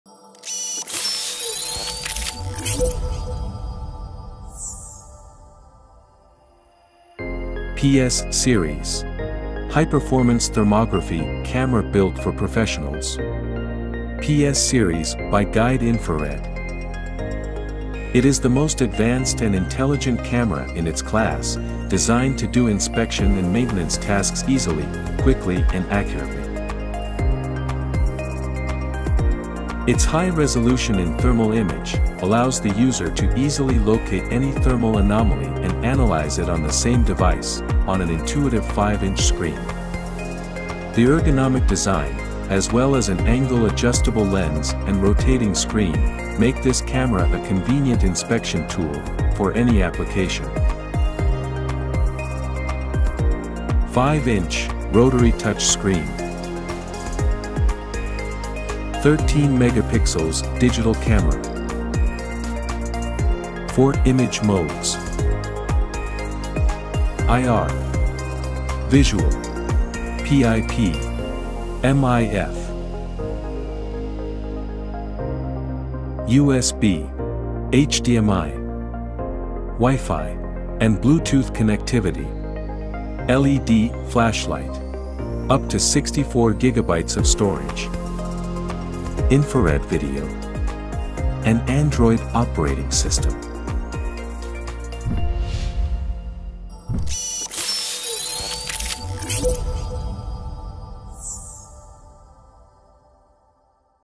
The Guide Sensmart PS Series high-performance thermal camera
The Guide PS Series high-performance thermal camera is designed to make the inspection, maintenance, and troubleshooting work easier, faster, and more accurate. It adopts a new generation of uncooled IR focal-plane detectors, which provides sharper thermal images and higher measurement accuracy.